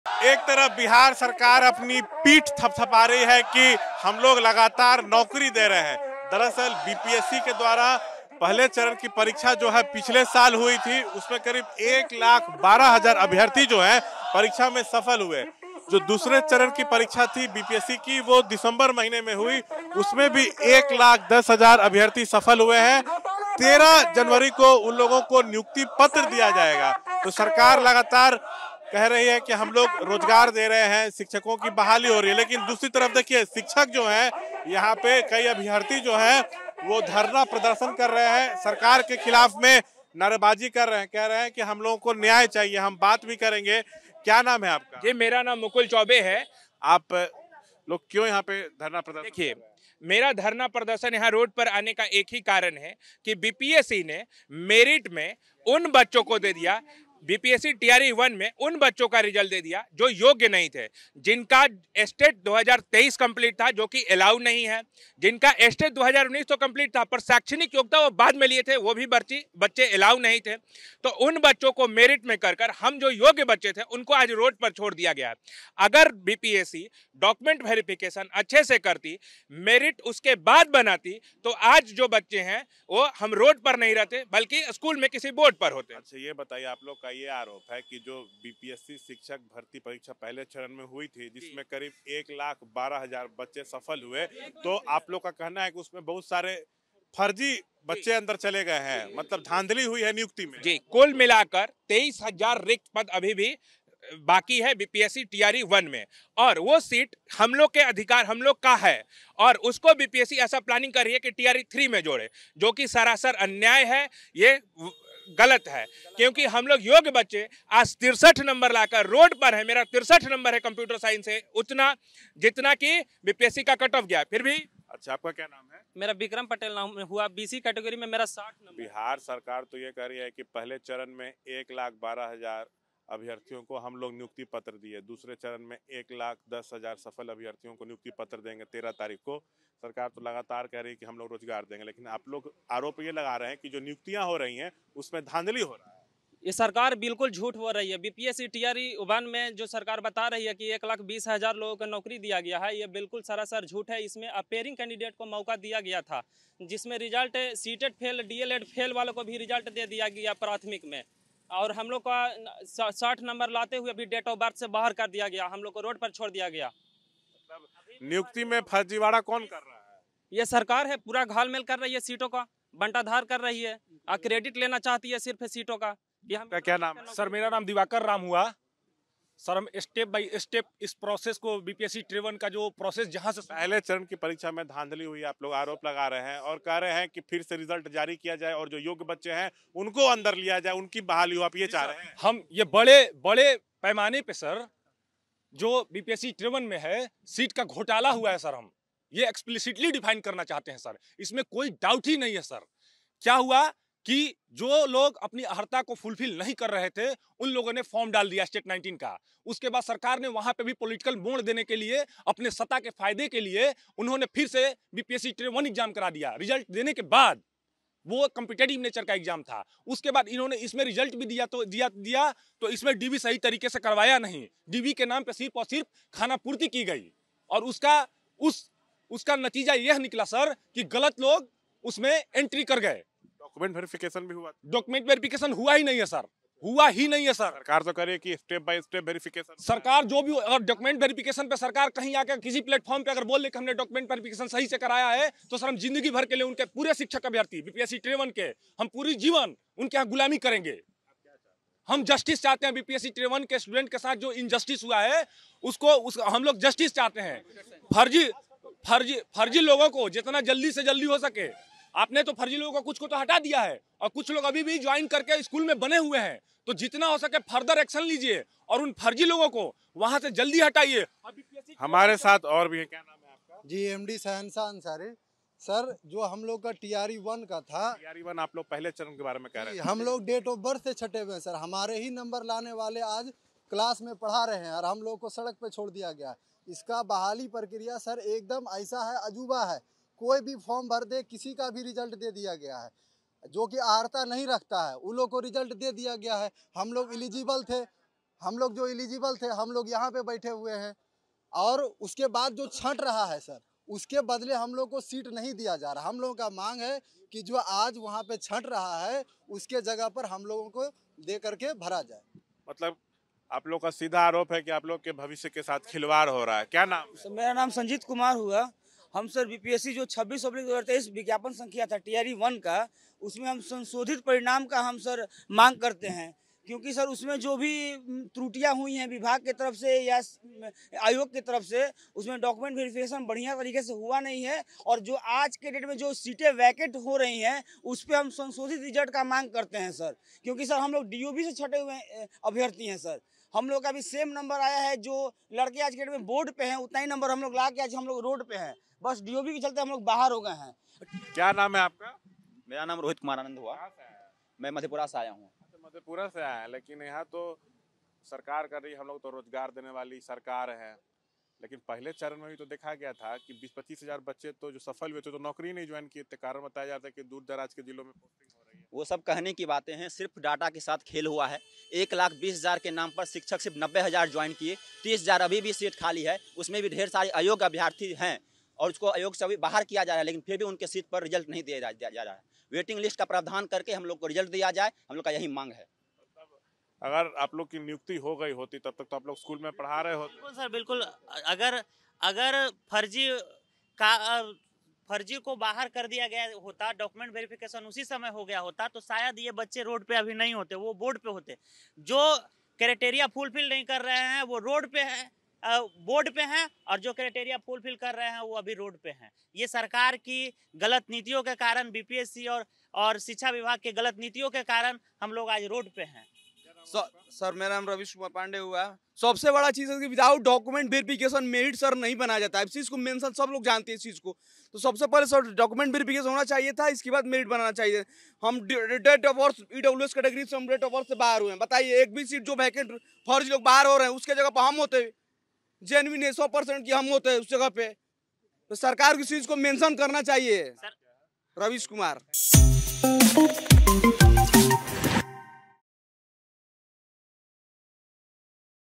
BPSC Protest: धरना-प्रदर्शन पर उतरे शिक्षक अभ्यर्थियों ने Bihar सरकार पर लगा दिए बड़े आरोप
पटना- शिक्षक अभ्यर्थी धरना प्रदर्शन कर रहे हैं. बिहार सरकार के खिलाफ नारेबाजी कर रहे हैं. दूसरी तरफ बिहार सरकार पीठ थपथपा रही है कि हम लोग शिक्षकों को नौकरी दे रहे हैं व नियुक्ति पत्र बांट रहे हैं .